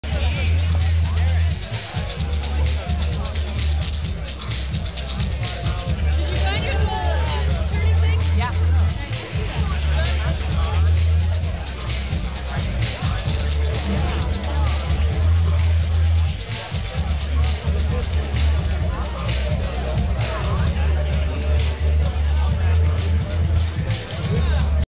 Red Rocks Rendezvous: Night Hooping with Erin & prAna
Nightime fun at Red Rocks Rendezvous.  Little E shows us how it's done with a hoop!